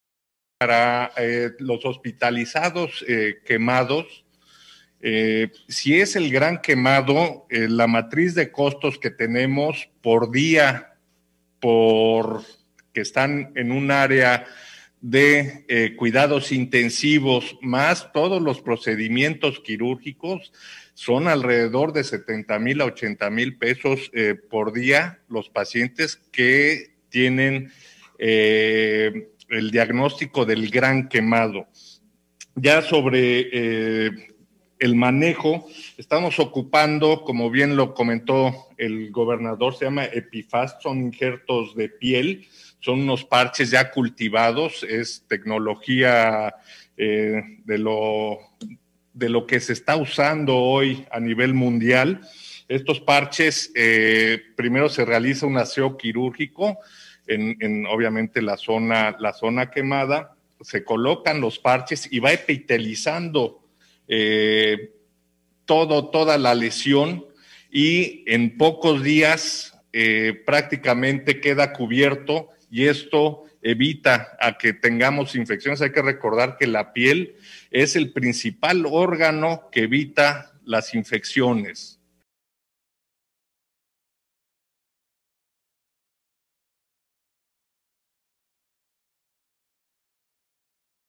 Atención hospitalaria a pacientes quemados de Xochimehuacan cuesta 80 mil pesos diarios
El secretario de Salud, José Martínez informó que dieron de alta a una mujer con quemaduras por la explosión en Xochimehuacan